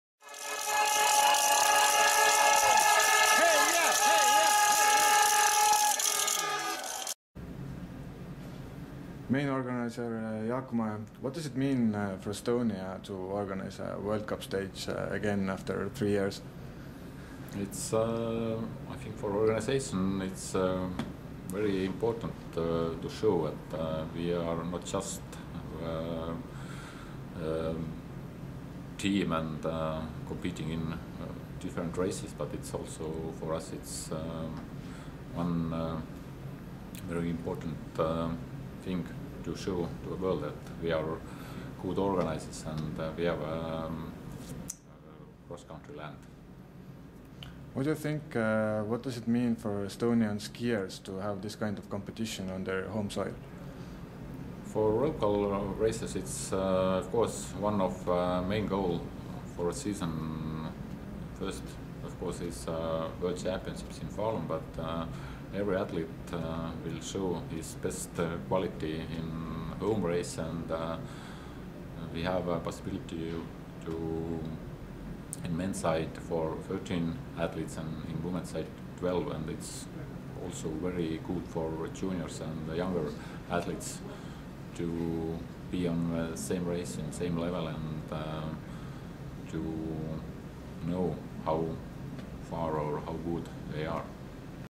Is it important for Estonia to have a World Cup stage again? Jaak Mae, Chief of LOC
Short interview with Jaak Mae.